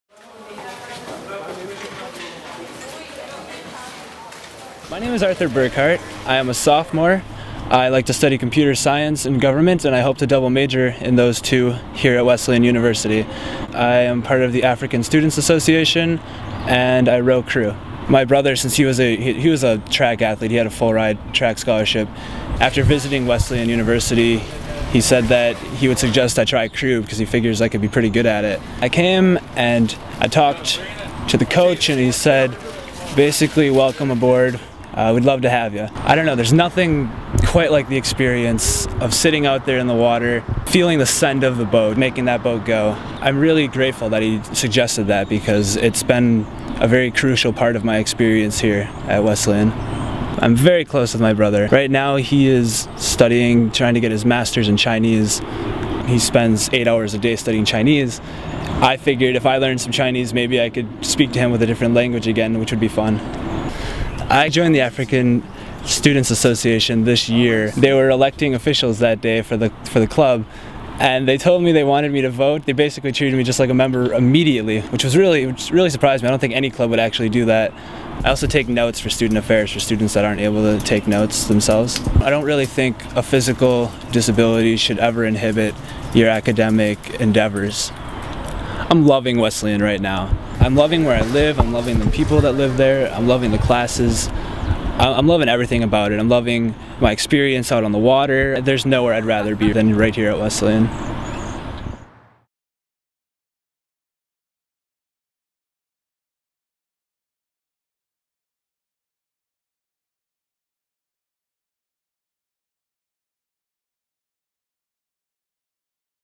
Arthur Burkart '14: The Wesleyan Student
The Wesleyan student is adventurous, passionate, and engaged with the world. Arthur Burkart '14 studies computer science and government, rows crew, and serves the student community in multiple ways. He volunteers taking notes for students who aren't able to take notes themselves.